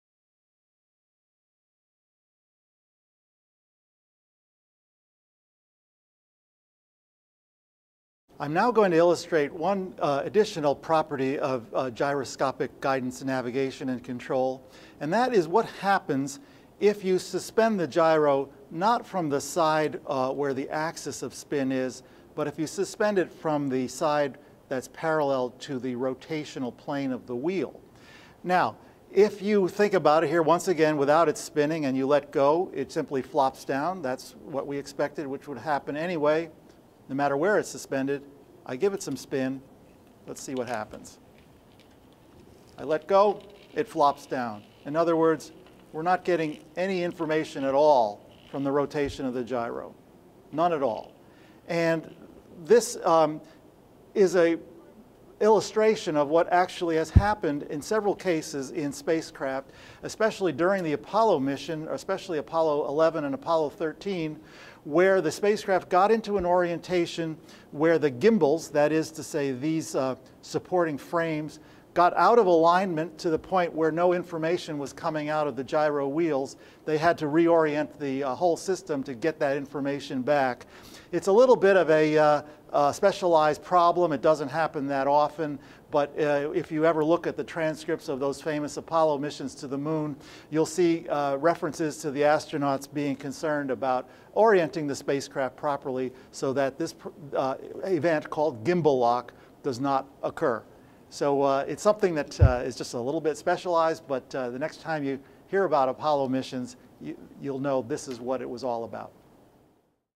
Gyroscopes and Gimbal Lock
Paul Ceruzzi, Chair of the Space History Department at the National Air and Space Museum, discusses gyroscopes and gimbal lock. For more information about navigating in space, please